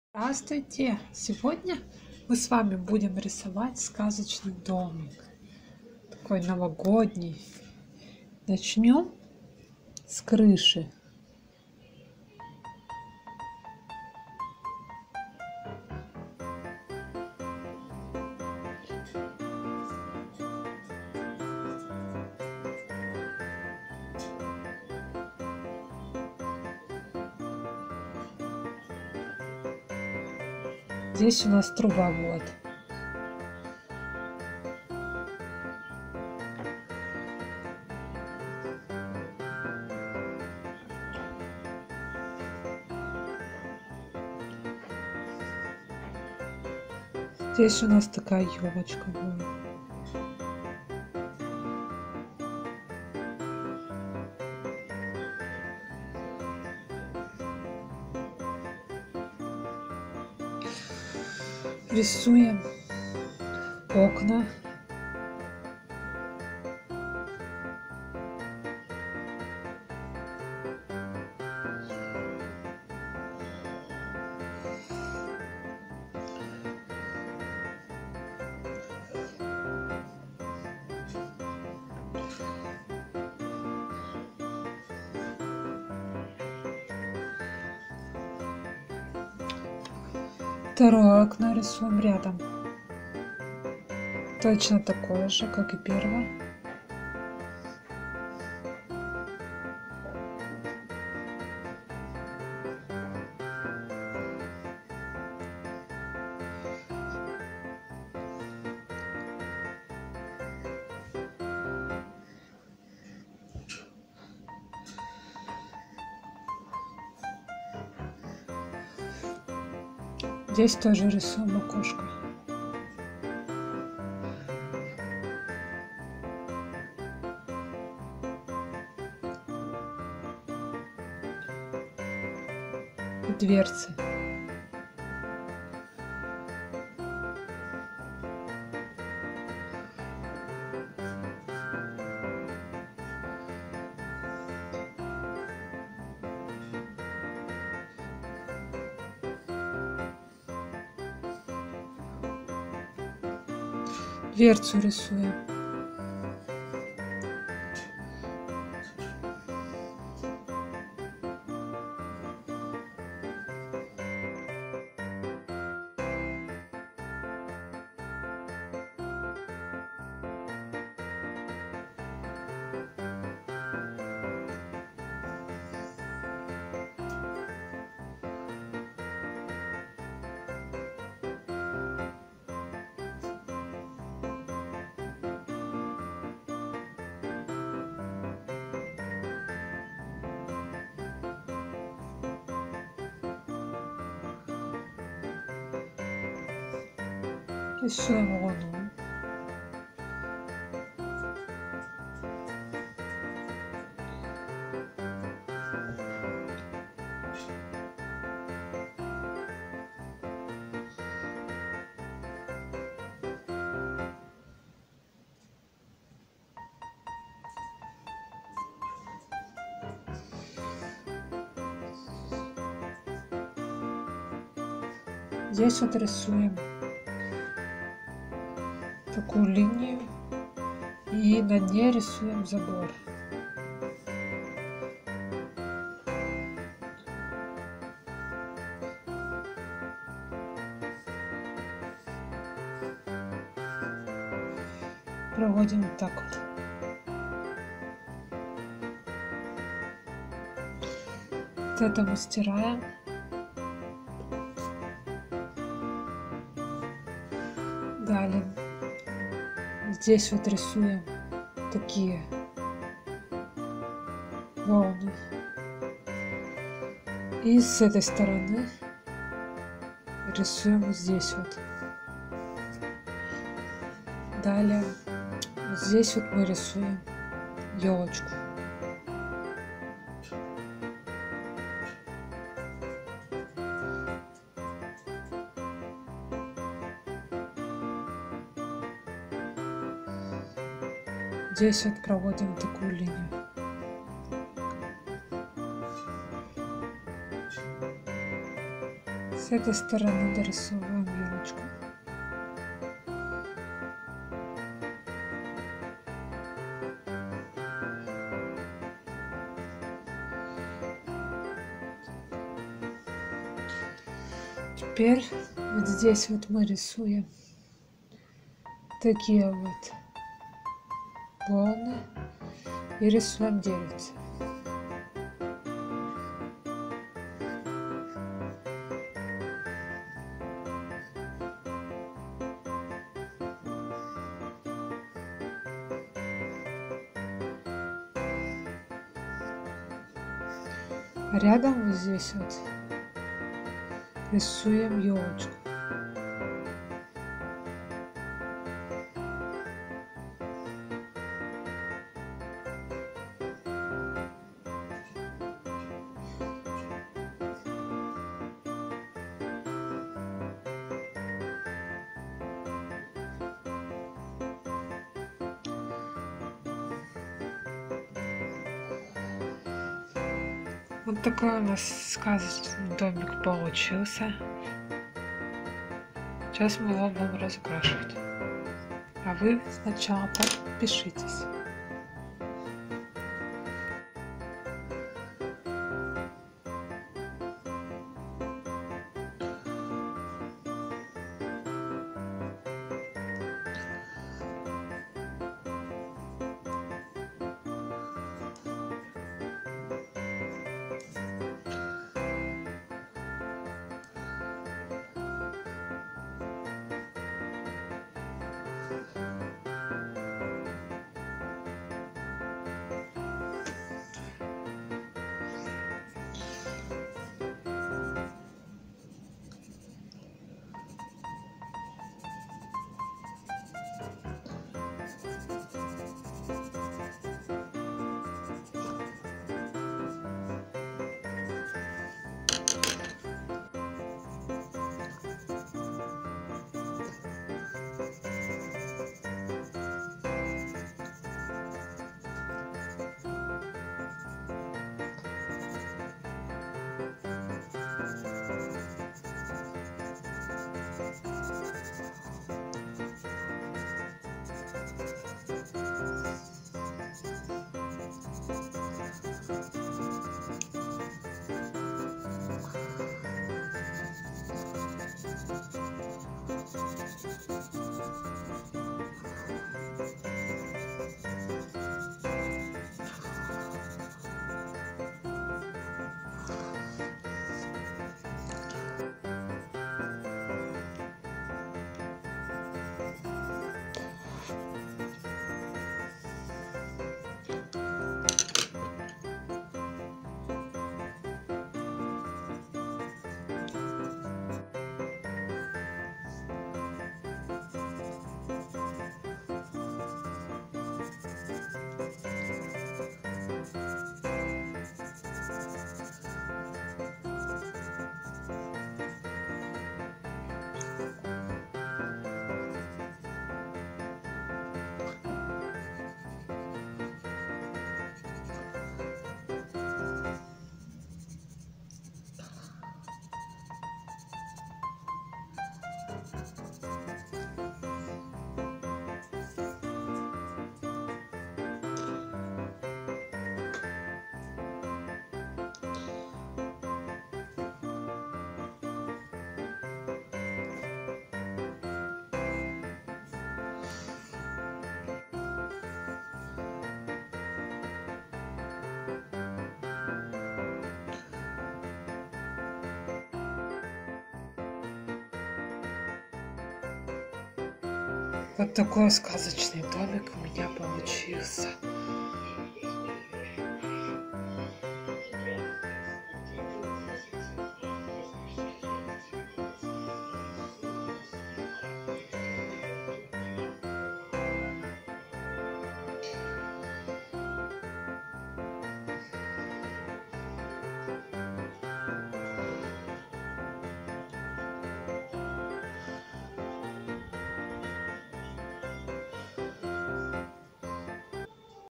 Влог / Нарисовать Новогодний Сказочный необычный домик / Нарисовать Дом Дед Мороза / Уроки Рисования
В этом видео как нарисовать Сказочный домик.